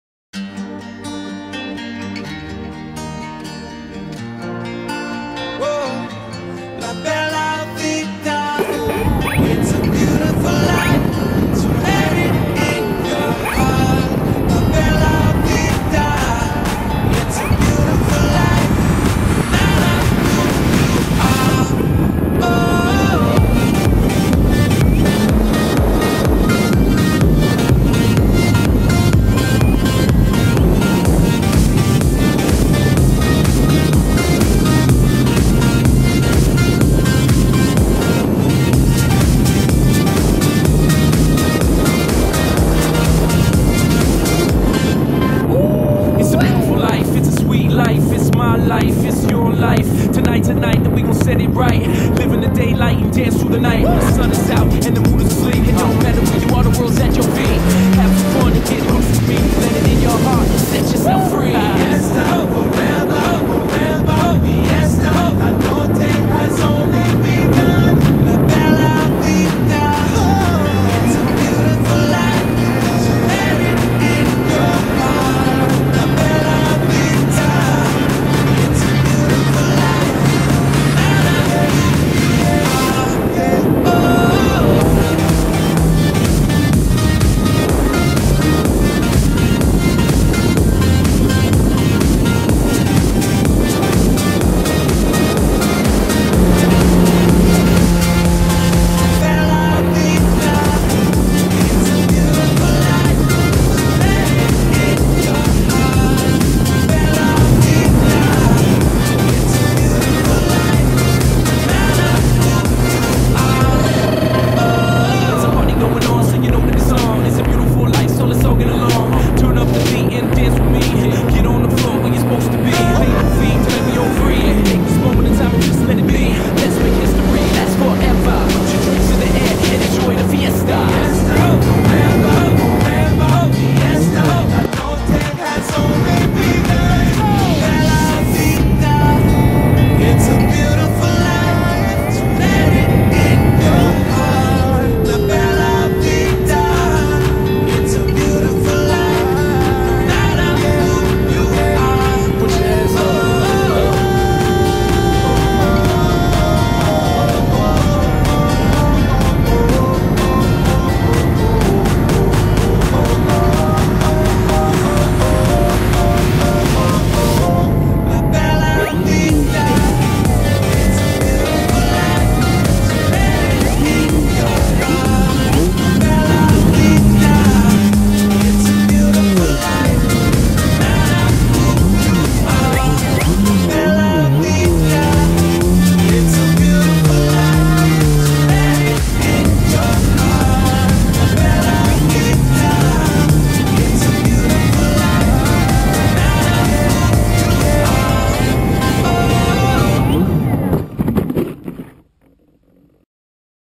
Acromaster + Go pro 01.02.2014 Flug über Breitbrunn an Ammersee
In diesem Video fliege ich mit dem Acromaster und einer Go Pro Hero 2 über Breitbrunn am Ammersee. Mit einigen kunstflugelementen